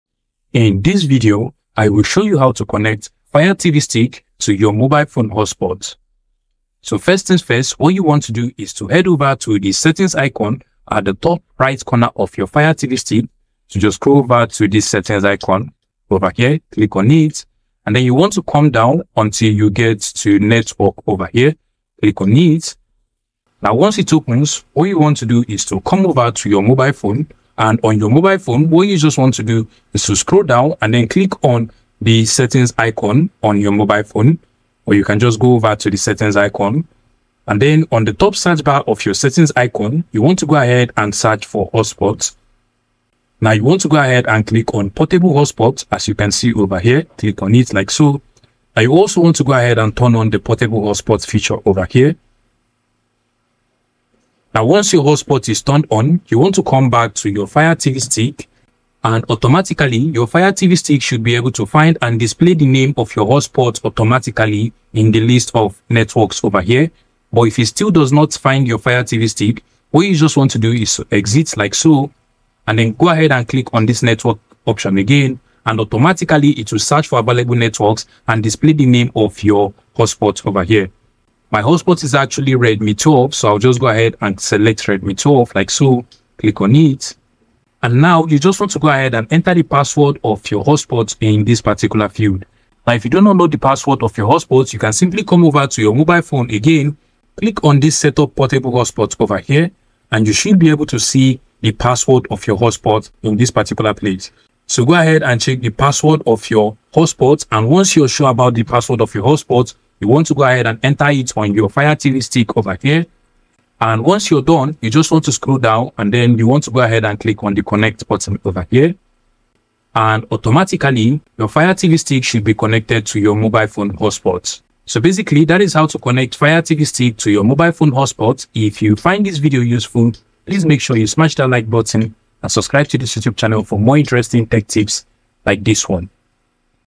How to Connect Fire TV Stick to Mobile Phone Hotspot
Learn How to Connect Fire TV Stick to Mobile Phone Hotspot in this step-by-step tutorial. In this video, I’ll show you how to connect your Fire TV Stick to mobile phone hotspot. It's a straightforward process that allows you to share your mobile phone internet with your smart TV using a hotspot. First, I will show you how to find your hotspot name and password. Turn on your mobile hotspot, then navigate to the connection settings on your Fire TV Stick. From there, you will learn how to search for available networks. Once you find your hotspot name, simply enter the password, and your Fire TV Stick will connect easily.

This easy tutorial works with all Fire TV and Fire tv sticks. Whether you're using a Fire TV or any Fire tv devices, this guide will help you get online quickly.